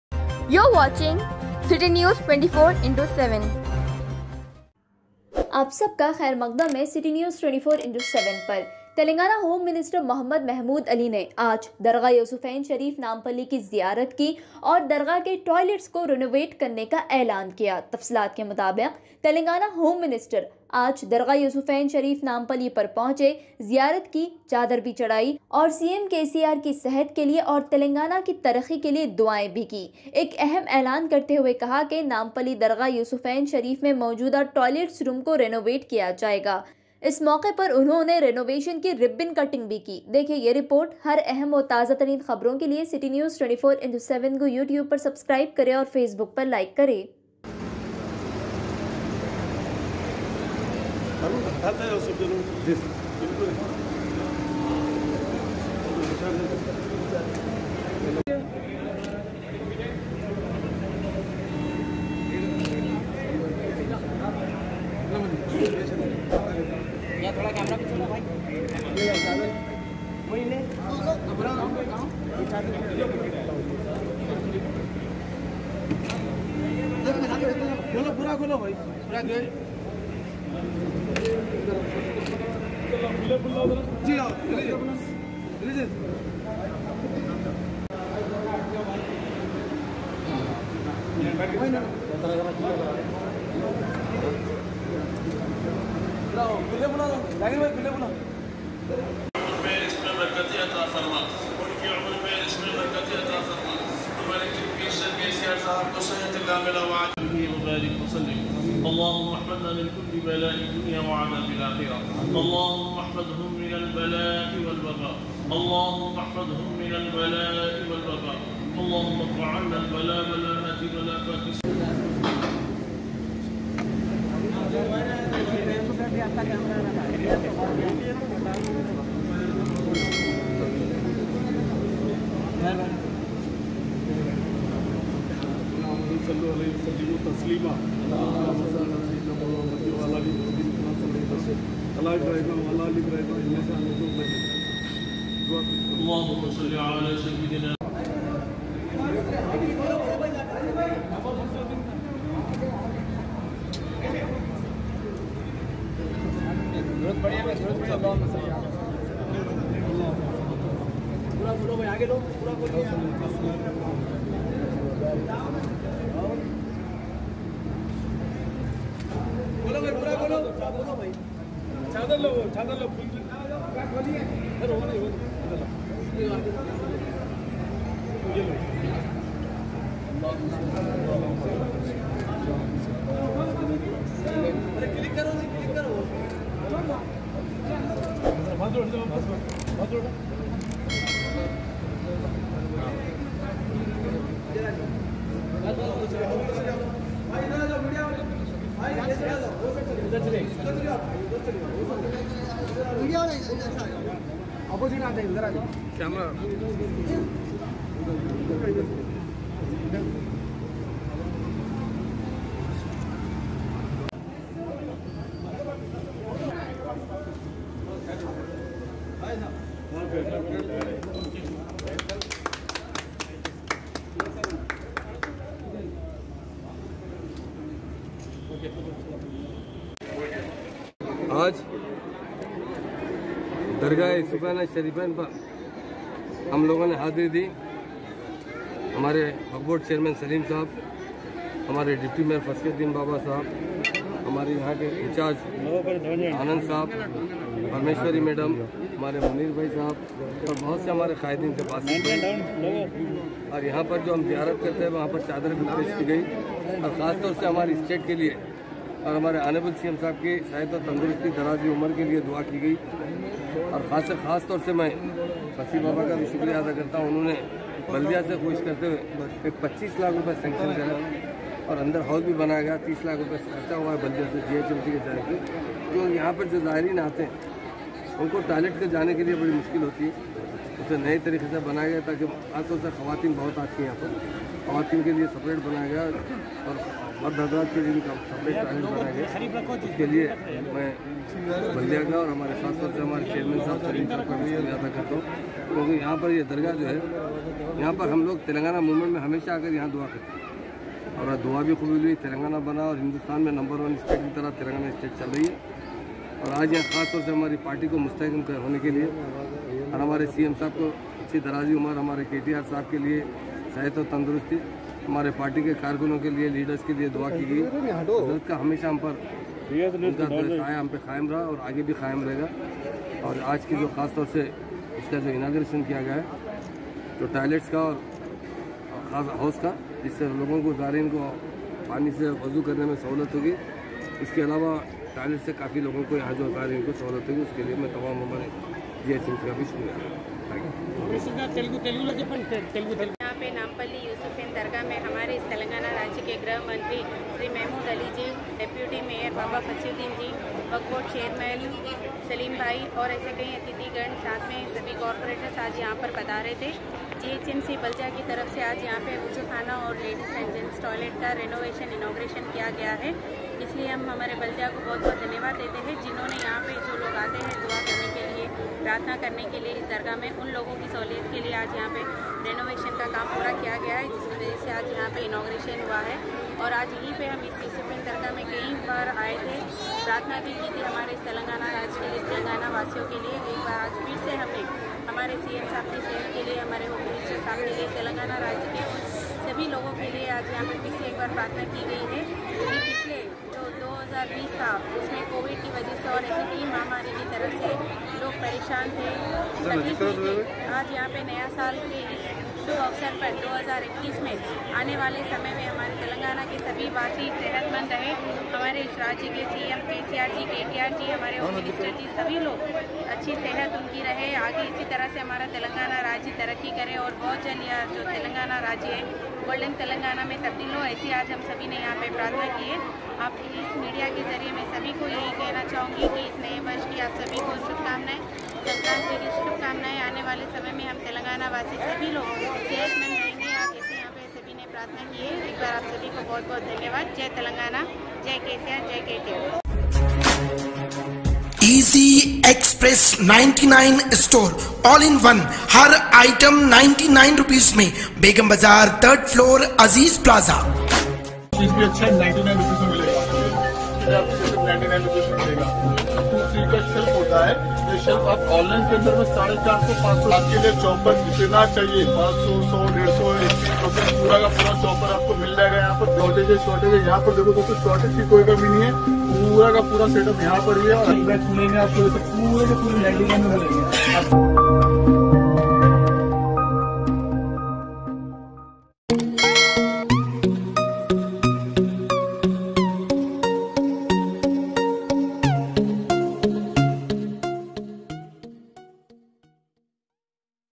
Nampally Dargah Mein Toilets Ka Renovation | Mehmood Ali | Md Saleem | Baba Fasi Uddin |@ CN24x7
Home Minister Mehmood Ali Visited Dargha Yousufain Shareef Nampally | INAUGURATION OF RENOVATION TOILETS

#| *REPORT BY :  SHER MOHAMMED KHAN  ||REPORTER OF *CITY_NEWS_24×7 ||.*